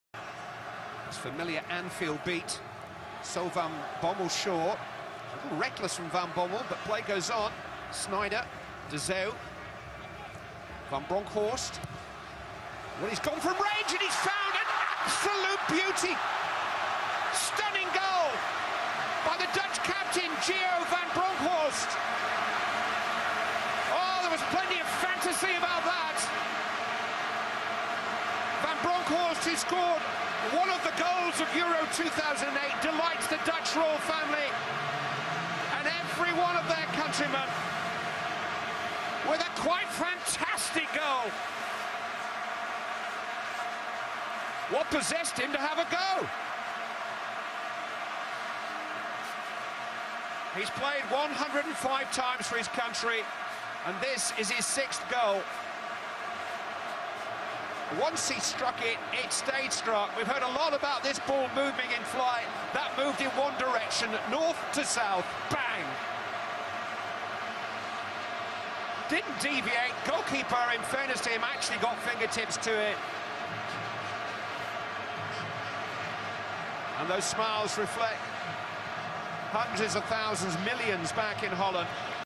Giovanni Van Bronckhorst long shot goal vs Uruguay
In this video we show you the amazing goal of Giovanni Van Bronckhorst vs Uruguay in the semi finals of the WC in 2010.

Who is Giovanni Van Bronckhorst?
- Giovanni Van Bronckhorst is now trainer of Feyenoord.
His clubs were;  RKC Waalwijk, Feyenoord, Glasgow Rangers, Arsenal and FC Barcelona.